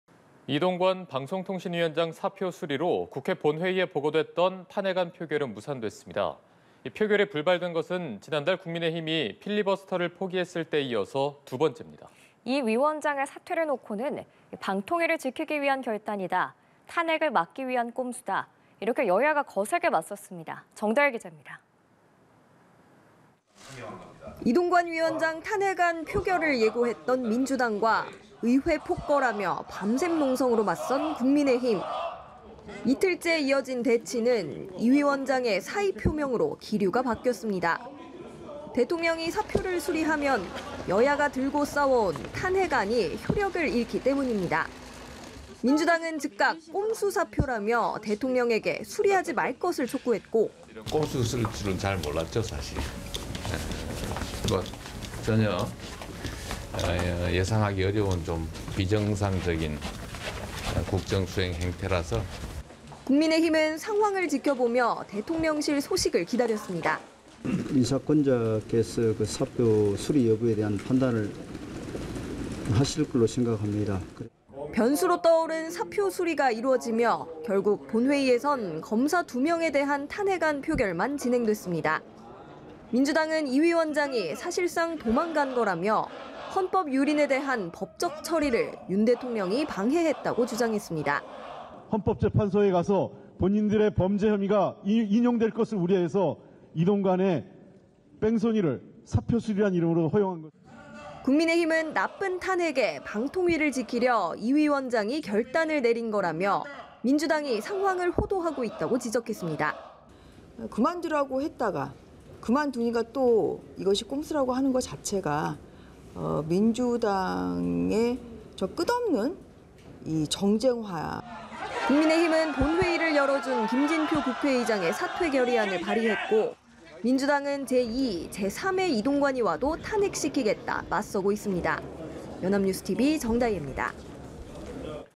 이동관 사퇴에 탄핵안 자동폐기…"방통위 지키는 결단" "꼼수 사퇴" / 연합뉴스TV (YonhapnewsTV)
이동관 사퇴에 탄핵안 자동폐기…"방통위 지키는 결단" "꼼수 사퇴"
[뉴스리뷰]

[앵커]

이동관 방송통신위원장 사표 수리로, 국회 본회의에 보고됐던 탄핵안 표결이 무산됐습니다.

표결이 불발된 것은 지난달 국민의힘이 필리버스터를 포기했을 때에 이어 두 번째입니다.

이 위원장의 사퇴를 놓고는 방통위를 지키기 위한 결단이다, 탄핵을 막기 위한 꼼수다, 여야가 거세게 맞섰습니다.

정다예 기자입니다.

[기자]

이동관 위원장 탄핵안 표결을 예고했던 민주당과, 의회 폭거라며 밤샘 농성으로 맞선 국민의힘.

이틀째 이어진 대치는, 이 위원장의 사의 표명으로 기류가 바뀌었습니다.

대통령이 사표를 수리하면, 여야가 들고 싸워온 탄핵안이 효력을 잃기 때문입니다.

민주당은 즉각 '꼼수 사표'라며 대통령에게 수리하지 말 것을 촉구했고,

[이재명 / 민주당 대표] "이런 꼼수 쓸 줄은 잘 몰랐죠. 사실, 전혀 예상하기 어려운 비정상적인 국정수행 형태…."

국민의힘은 상황을 지켜보며 대통령실 소식을 기다렸습니다.

[윤재옥 / 국민의힘 원내대표] "인사권자께서 사표 수리 여부에 대한 판단을 하실 걸로…."

변수로 떠오른 사표 수리가 이뤄지며, 결국 본회의에선 검사 2명에 대한 탄핵안 표결만 진행됐습니다.

민주당은 이 위원장이 사실상 도망간 거라며, 헌법 유린에 대한 법적 처리를 윤 대통령이 방해했다고 주장했습니다.

[홍익표 / 민주당 원내대표] "헌법재판소에서 본인들의 범죄 혐의가 인용될 것을 우려해서 이동관의 뺑소니를 사표 수리란 이름으로 허용…."

국민의힘은 '나쁜 탄핵'에 방통위를 지키려 이 위원장이 결단을 내린 거라며, 민주당이 "상황을 호도하고 있다"고 지적했습니다.

[전주혜 / 국민의힘 원내대변인] "그만두라고 했다가 그만두니까 그것이 꼼수라는 것 자체가 민주당의 끝없는 정쟁화…."

국민의힘은 본회의를 열어준 김진표 국회의장의 사퇴 결의안을 발의했고, 민주당은 제2, 제3의 이동관이 와도 탄핵시키겠다, 맞서고 있습니다.